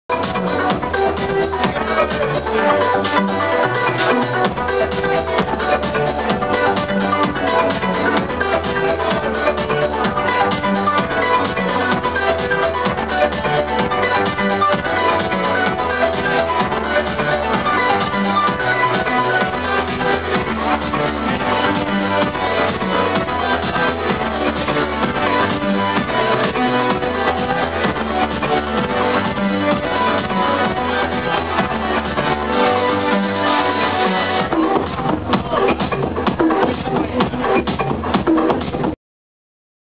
Gabriel & Dresden @ Pawn Shop WMC 2008 Pt 5
Gabriel & Dresden playing live at Pawn Shop in Miami, FL Friday night.